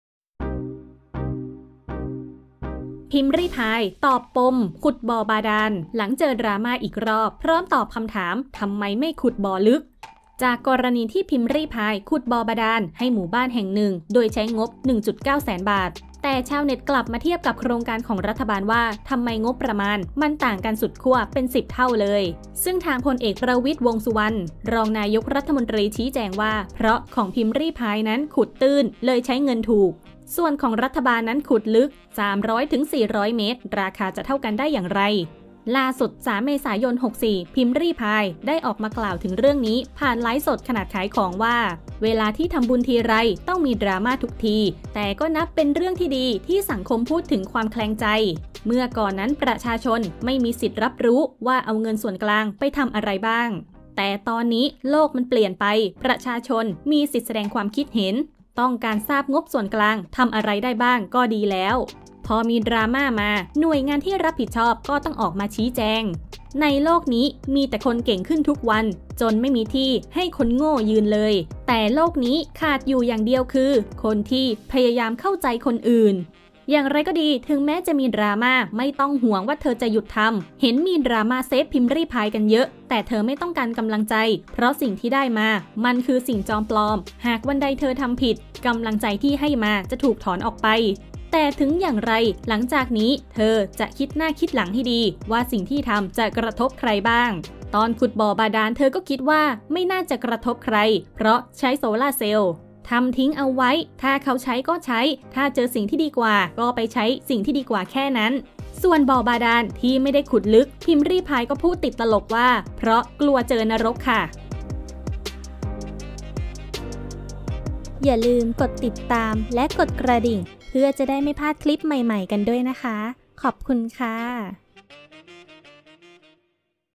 พิมรี่พาย ตอบปมขุดบ่อบาดาล ฟาดกลับเจ็บๆ เหตุผลที่ไม่ได้ขุดบ่อลึกๆ
พิมรี่พาย ฟาดกลับเจ็บๆ เหตุผลที่ไม่ได้ขุดลึก

 จากกรณีที่พิมรี่พายขุดบ่อบาดาลให้หมู่บ้านแห่งหนึ่ง โดยใช้งบ 1.9 แสนบาท แต่ชาวเน็ตกลับมาเทียบกับโครงการของรัฐบาลว่า ทำไมงบประมาณมันต่างกันสุดขั้วเป็น 10 เท่าเลย ซึ่งทาง พล.อ. ประวิตร วงษ์สุวรรณ รองนายกรัฐมนตรี ชี้แจงว่า เพราะของพิมรี่พายนั้นขุดตื้น เลยใช้เงินถูก ส่วนของรัฐบาลขุดลึก 300-400 เมตร ราคาจะเท่ากันได้อย่างไร

ล่าสุด (3 เมษายน 2564) พิมรี่พาย ได้ออกมากล่าวถึงเรื่องนี้ผ่านไลฟ์ขณะขายของว่า เวลาที่ทำบุญทีไรต้องมีดราม่าทุกที แต่ก็นับเป็นเรื่องที่ดีที่สังคมพูดถึงความแคลงใจ เมื่อก่อนนั้นประชาชนไม่มีสิทธิ์รับรู้ว่าเอาเงินส่วนกลางไปทำอะไรบ้าง แต่ตอนนี้โลกมันเปลี่ยนไป ประชาชนมีสิทธิ์แสดงความคิดเห็น ต้องการทราบงบส่วนกลางทำอะไรได้บ้าง ก็ดีแล้ว พอมีดราม่าหน่วยงานที่รับผิดชอบก็ต้องออกมาชี้แจง

           ในโลกนี้มีแต่คนเก่งขึ้นทุกวันจนไม่มีที่ให้คนโง่ยืนเลย แต่โลกนี้ขาดอยู่อย่างเดียว คือ คนที่พยายามเข้าใจคนอื่น

อย่างไรก็ดี แม้จะมีดราม่า ไม่ต้องห่วงว่าเธอจะหยุดทำ เห็นมีดราม่าเซฟพิมรี่พายกันเยอะ แต่เธอไม่ต้องการกำลังใจ เพราะสิ่งที่ได้มามันคือสิ่งจอมปลอม หากวันใดเธอทำผิด กำลังใจที่ให้มาก็จะถูกถอนออกไป แต่ถึงอย่างไร หลังจากนี้เธอจะคิดหน้าคิดหลังให้ดีว่าสิ่งที่ทำจะกระทบกับใครบ้าง ตอนขุดบ่อบาดาลเธอก็คิดว่า ไม่น่าจะกระทบกับใคร เพราะใช้โซลาร์เซลล์ ทำทิ้งเอาไว้ ถ้าเขาใช้ก็ใช้ ถ้าเจอสิ่งที่ดีกว่า ก็ไปใช้สิ่งที่ดีกว่าแค่นั้น

           ส่วนบ่อบาดาลที่ไม่ได้ขุดลึก พิมรี่พายก็พูดติดตลกว่า "เพราะกลัวเจอนรกค่ะ"